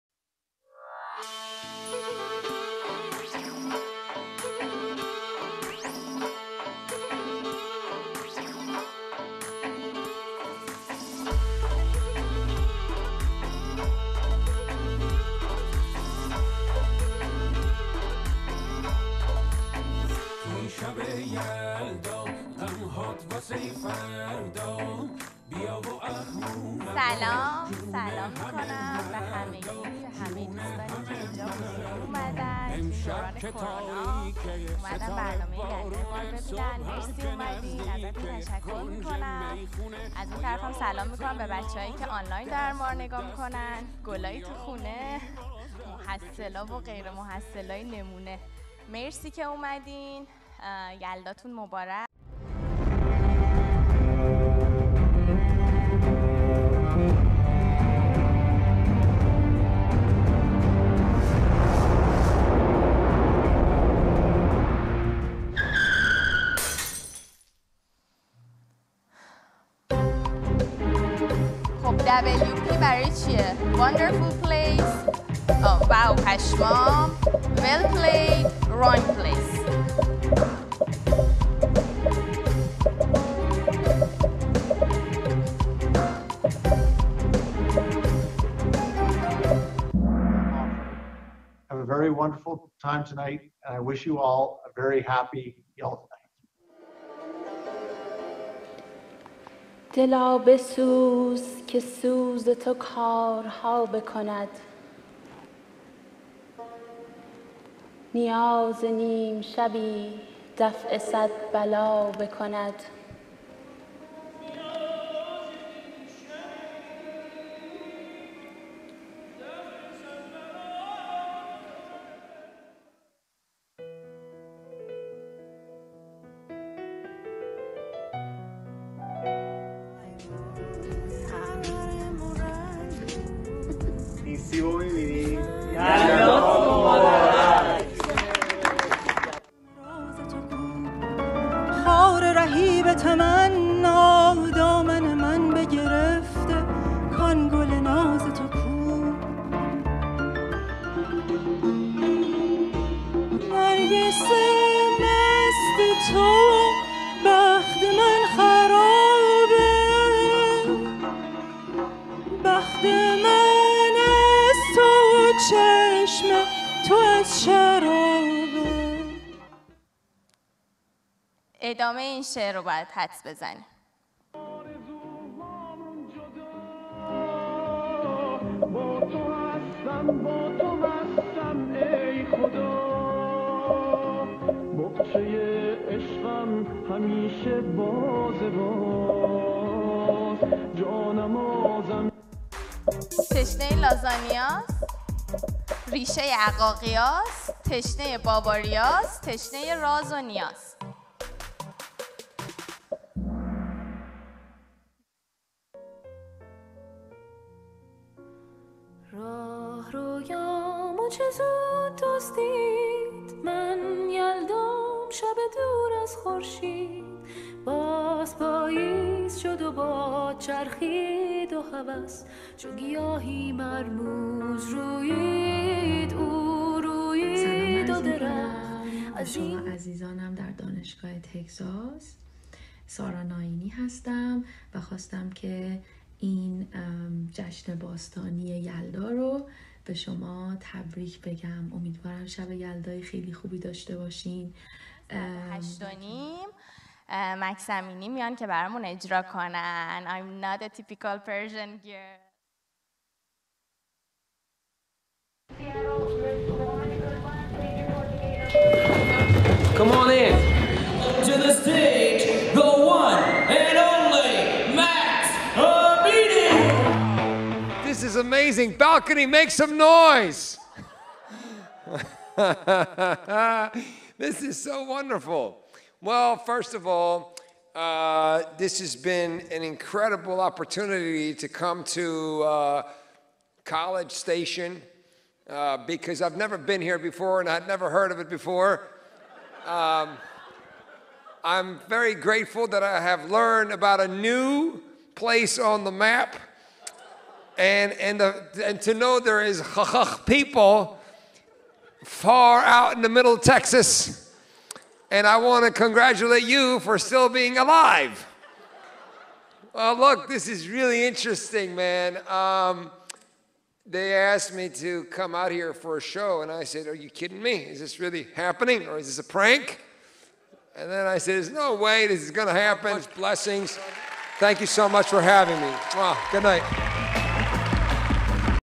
Yalda Night Ceremony 1399
This is a summary of Yalda Night Ceremony held on December 12, 2020 at Rudder Auditorium of Texas A&M University. In this event, we had different kinds of performances such as music, reading poems of Hafez, stand-up comedy with Max Amini along with some video clips and games.
Due to the Covid-19, in this event, social distancing was practiced and the live stream option was offered through Zoom application.